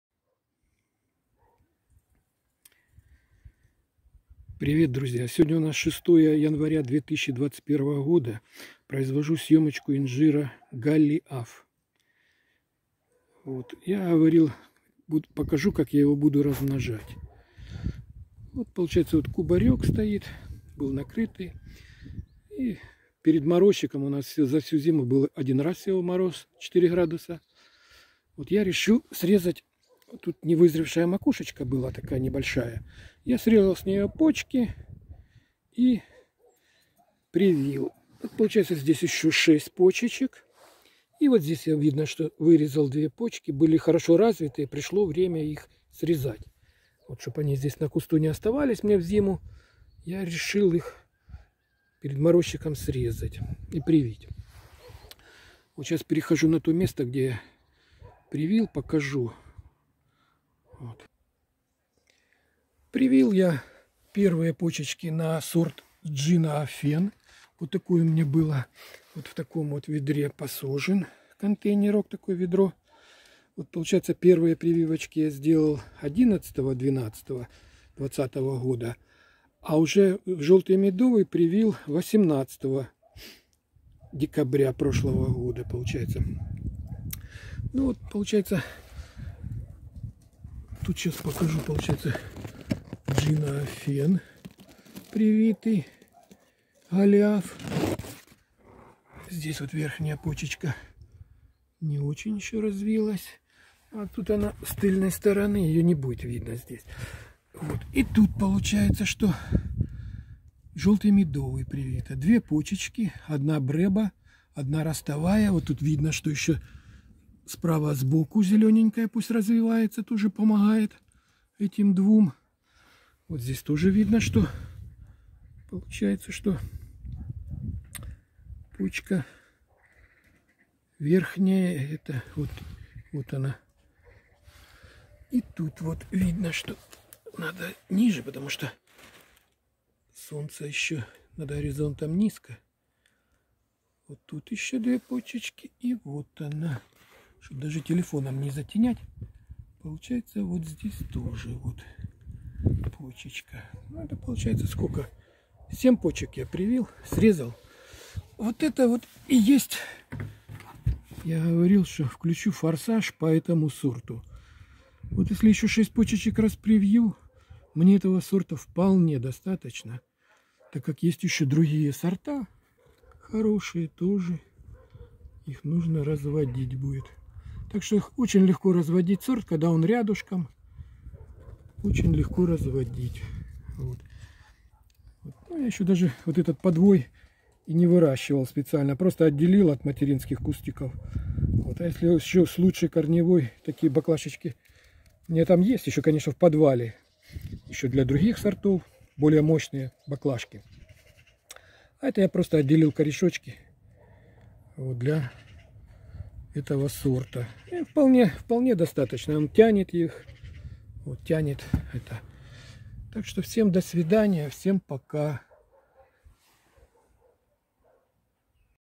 Инжир Голиаф.Размножение.
В видео показано как быстро размножить любой сорт на собственном подвое.Привитый инжир развивается на подоконнике у окна.